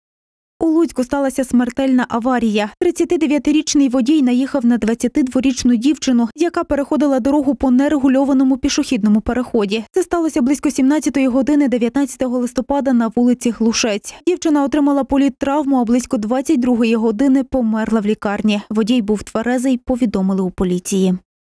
Збив дівчину у Луцьку - померла у лікарні. Їй було 22.
У Луцьку сталася смертельна аварія. 39-річний водій наїхав на 22-річну дівчину, яка переходила дорогу по нерегульованому переходу. Це сталося близько 17-ї години 19 листопада на вулиці Глушець. Дівчина отримала політравму, а близько 22-ї години, померла в лікарні. Водій був тверезий, повідомили у поліції.
12 Канал у телеграмі, усі важливі новини Волині, України і світу.